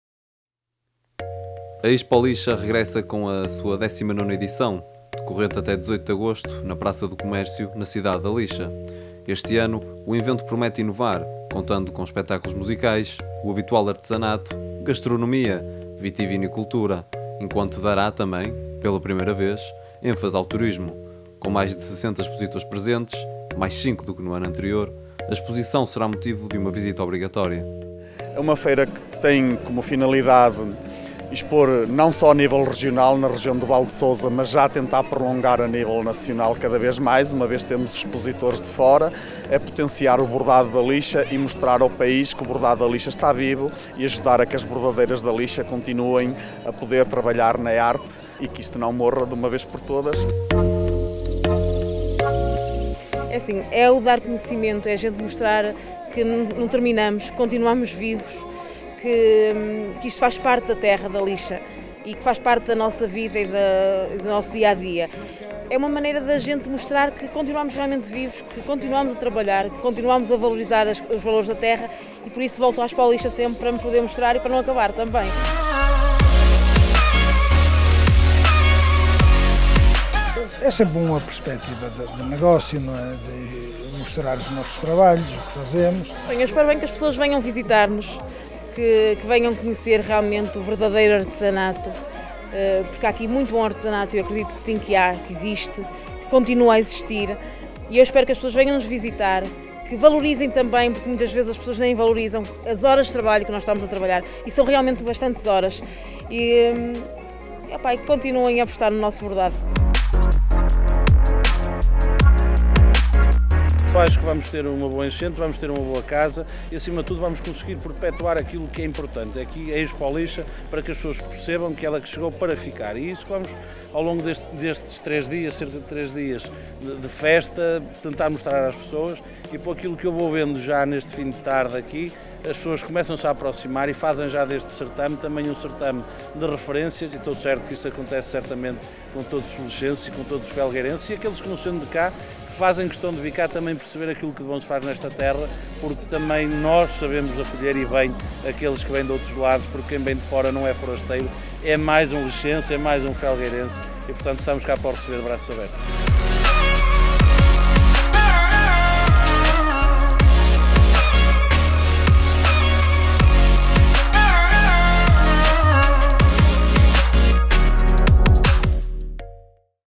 A cidade da Lixa acolhe mais uma edição da EXPOLIXA
A Expolixa regressa com a sua 19º edição, decorrente até 18 de agosto na Praça do Comércio, na cidade da Lixa.
Este ano o evento promete inovar, contando com espetáculos musicais, o habitual artesanato, gastronomia, vitivinicultura enquanto dará também, pela primeira vez, ênfase ao turismo. Com mais de 60 expositores presentes, mais 5 do que no ano anterior, a exposição será motivo de uma visita obrigatória.